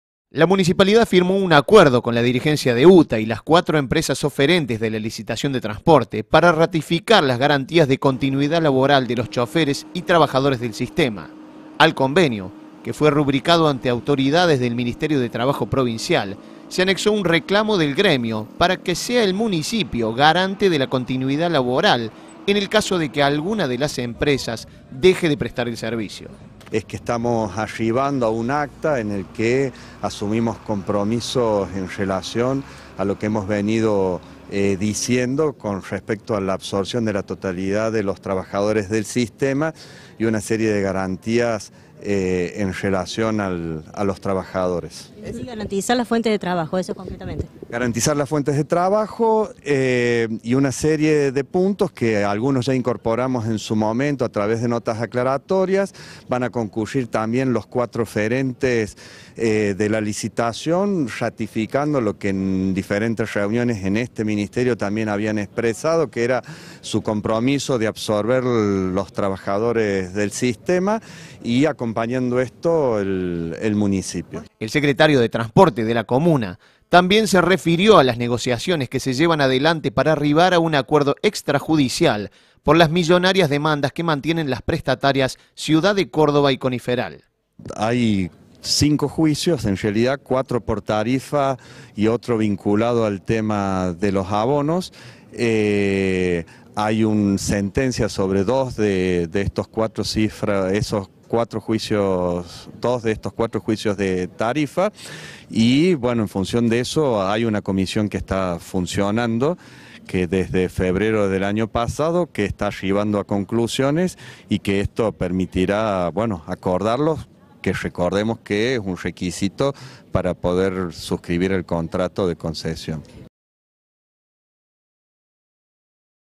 Acuerdo entre la Municipalidad, UTA y las empresas para garantizar empleo a los choferes
Lo indicó el secretario de Transporte municipal, al señalar que se garantizará la fuente de trabajo, como se indicó antes.